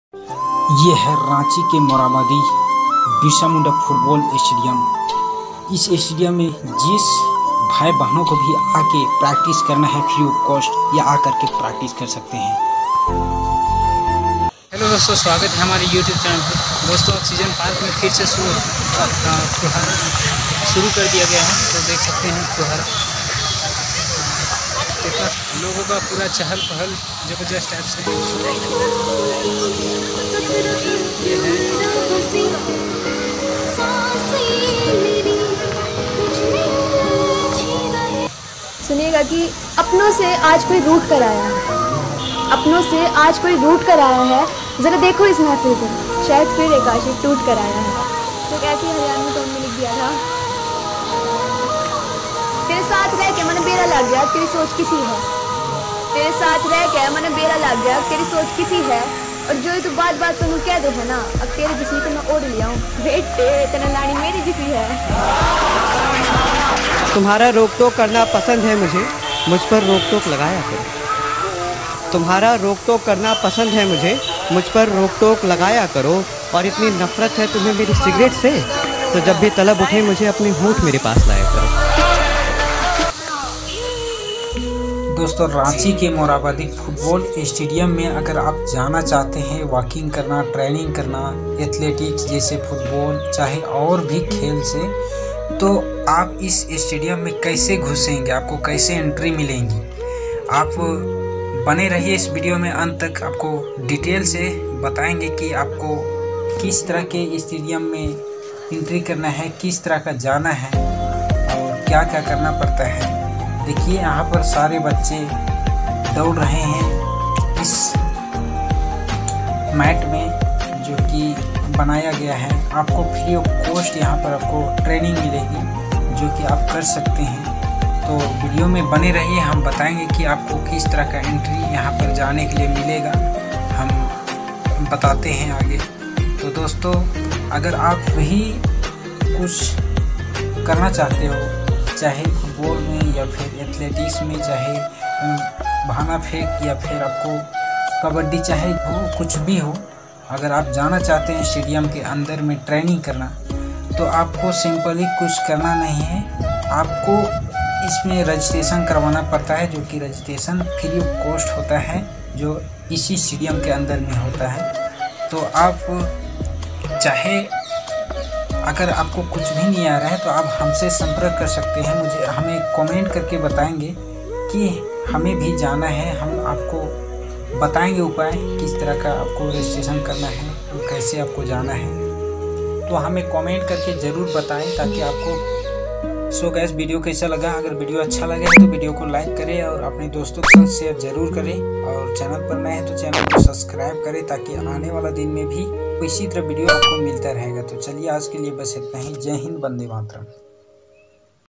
RANCHI KE morhabadi Stadium ban kr tyar ho chuka hi,आप भी ईस स्टेडियम में जा सकते हैं।
janiye kaise ja sakte hi